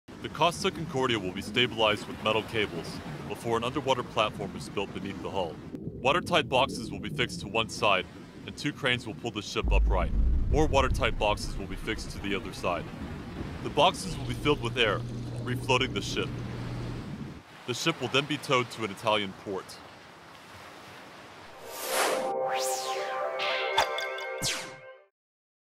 Costa Concordia salvage operation begins
Salvage work has begun on the stricken cruise ship Costa Concordia.

The 115,000 ton ship sank off the coast of the Italian island of Giglio in January with the loss of 32 lives. The four-stage project is estimated to take around one year. SOURCES: BBC